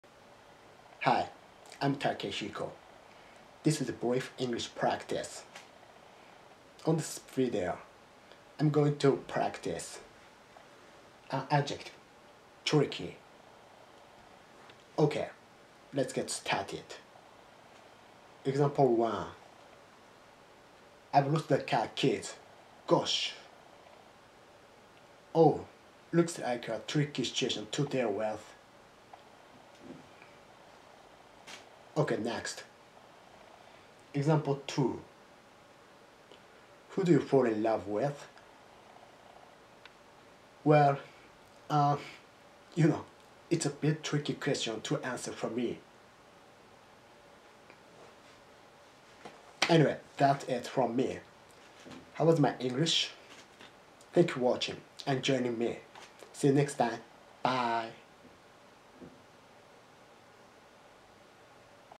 brief English practice, "tricky"
On this video, I'm practicing "tricky".